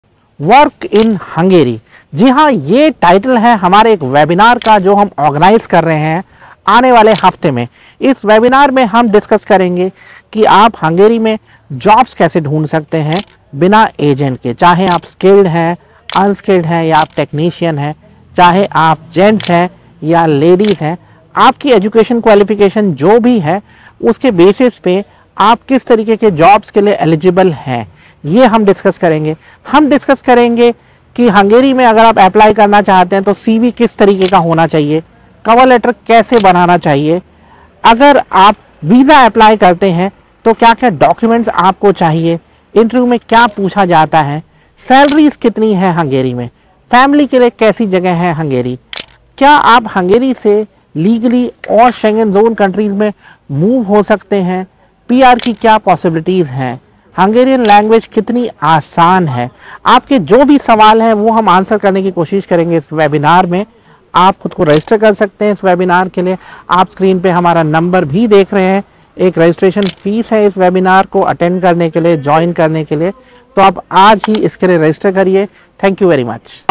Work in Hungary Webinar
Do you want to work in Hungary ? We are organising a Webinar where we answer your questions about
How to find jobs in Hungary ?
What are the jobs available?
What is the salary ?
How much time it takes to get visa ?
How to prepare documents for visa ?
Visa interview
Do join our webinar

Chandra Shekher sir has been guiding students and other about Studying Abroad, Settling Abroad or Travelling Abroad with his first hand experience of having lived and studied in the US, UK and Europe.